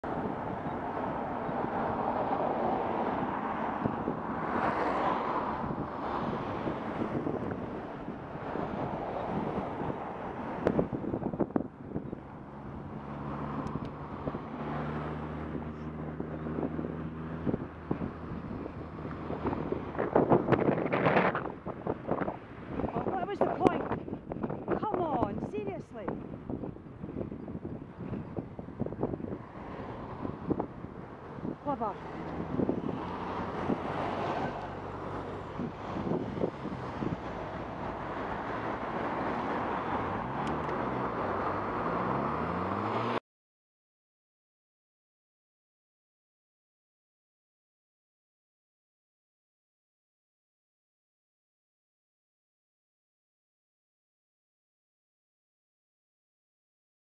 Close pass merc and mgif Yaris W727 NSF
I felt the merc was a bit too close for comfort the cars after were not too bad but the yaris driver takes the biscuit. He was braking even before he was past me. It got him far eh. Little unrestrained dog as well.